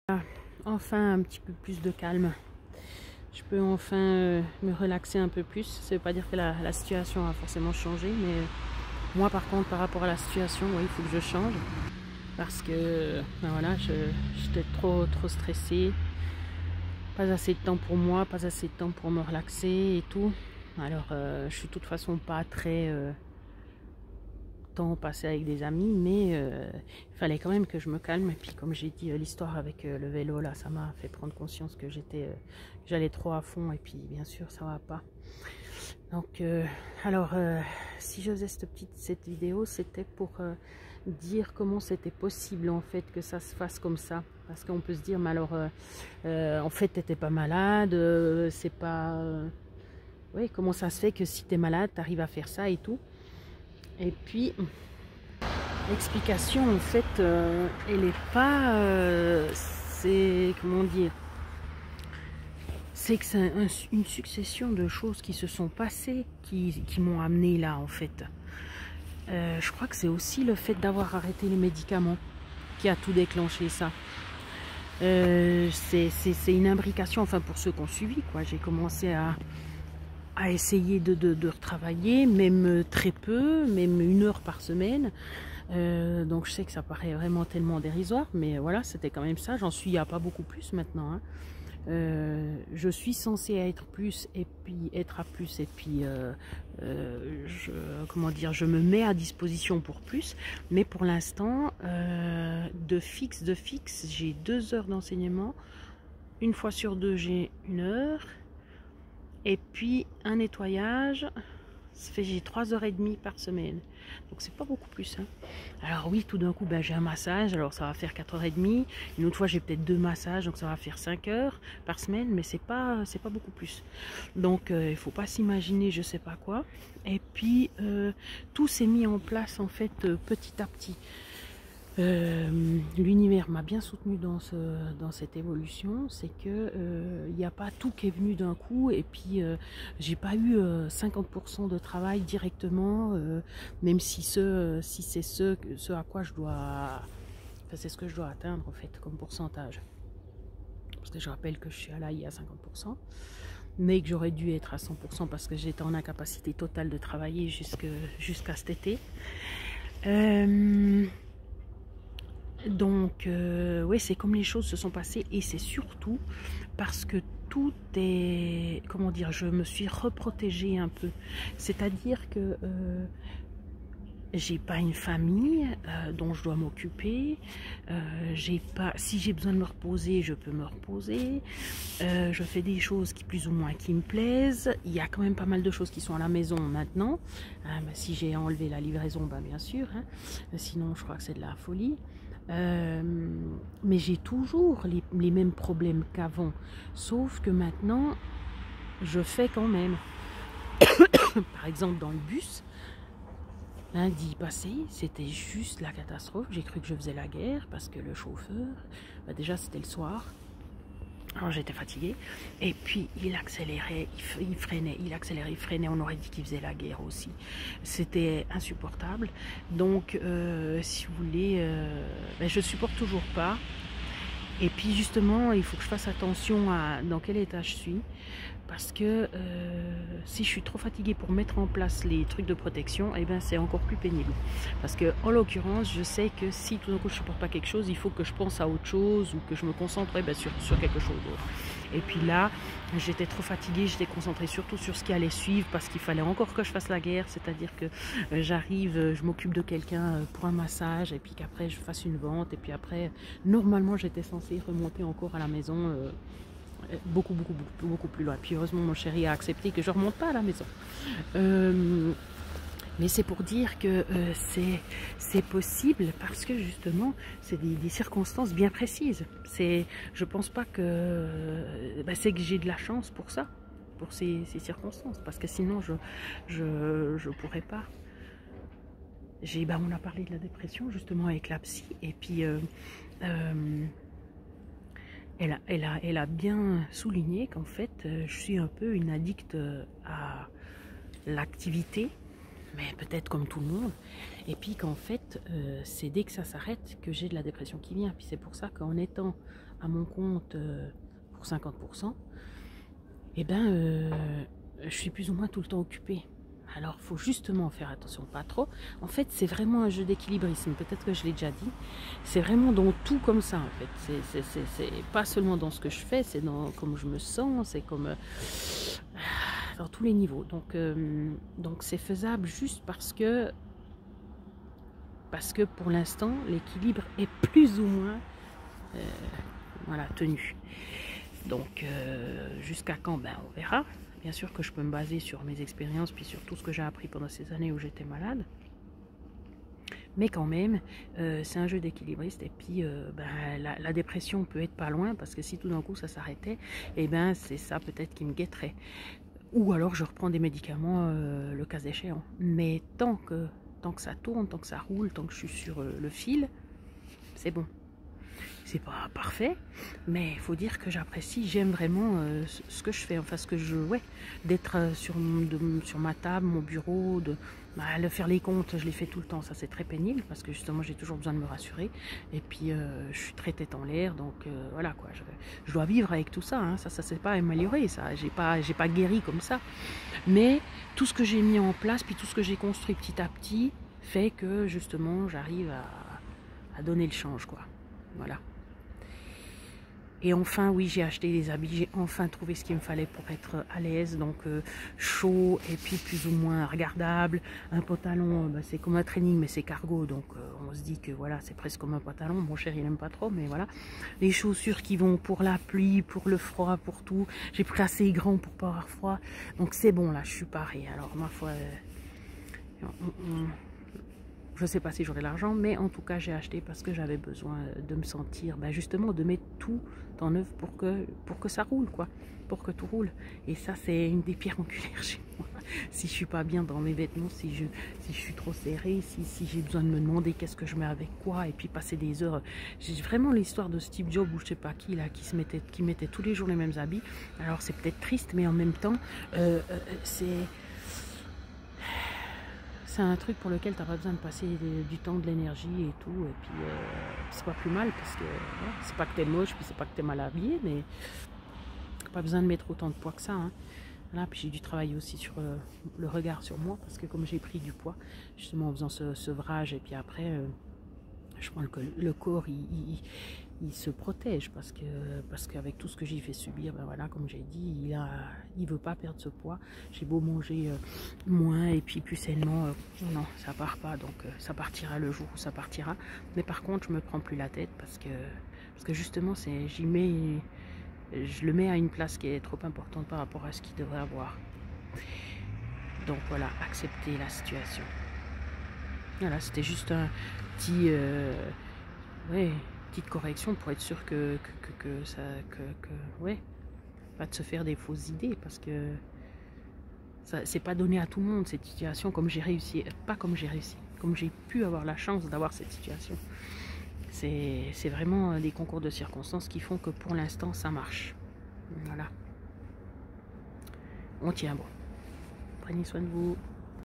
Sans médicaments : oui mais comment est-ce possible
ca s'est construit petit à petit, je crois en l'Univers, que c'est possible, je me donne la chance d'y arriver, et surtout j'ai les conditions qui sont réunies ou que je réunis (je me construis un monde)